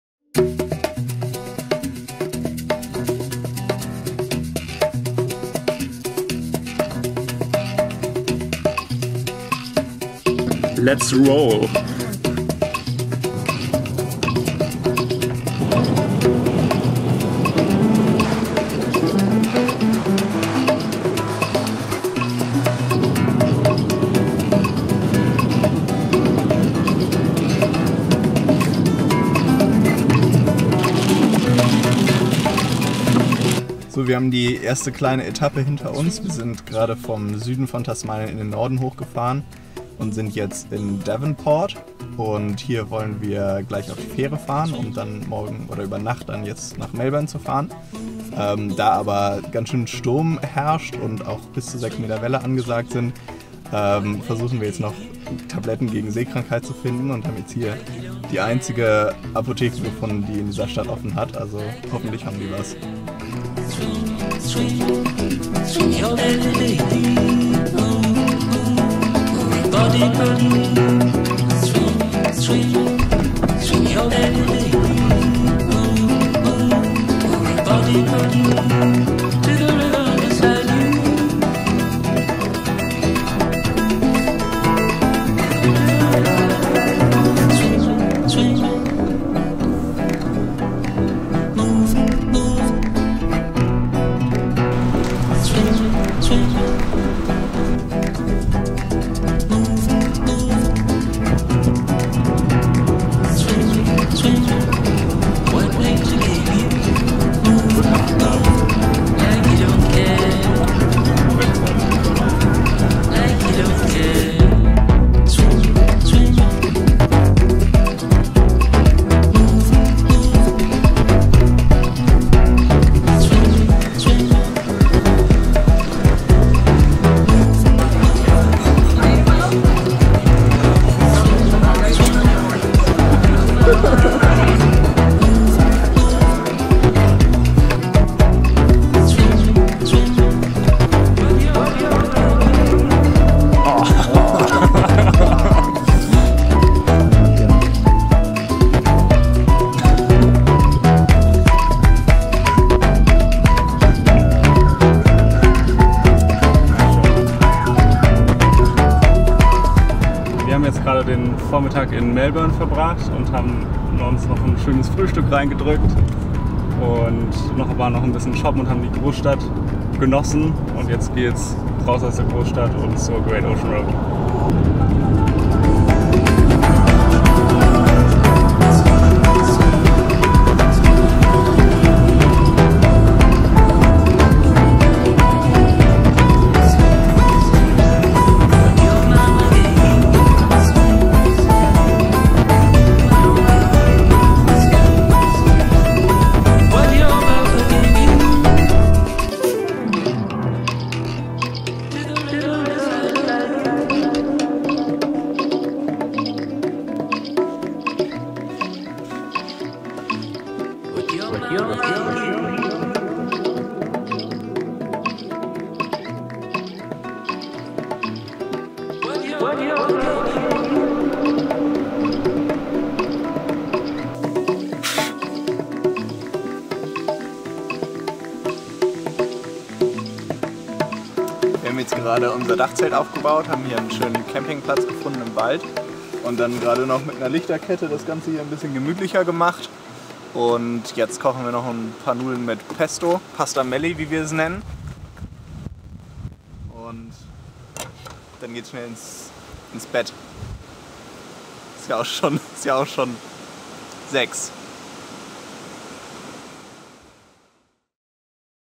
Tschüss Tassie! - Australien Roadtrip - Folge 1
Die erste Folge von unserem kleinen Vlog Projekt in dem wir unseren Roadtrip von Tasmanien nach Perth dokumentieren.